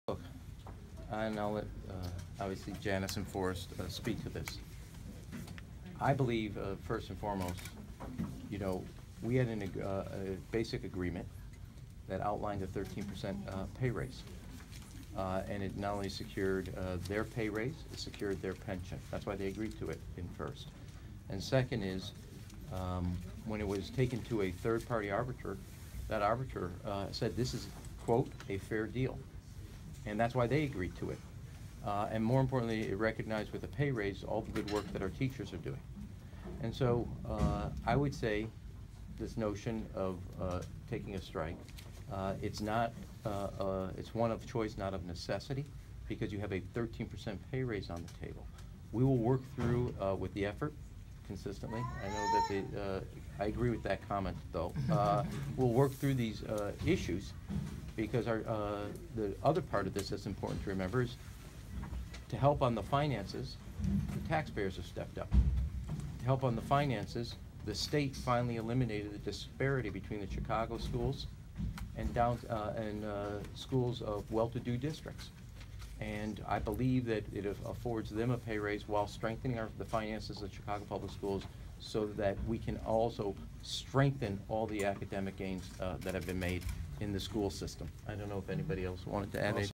Mayor Emanuel on possible teachers' strike
Mayor Rahm Emanuel talked about the possibility of a strike Thursday at an unrelated news conference.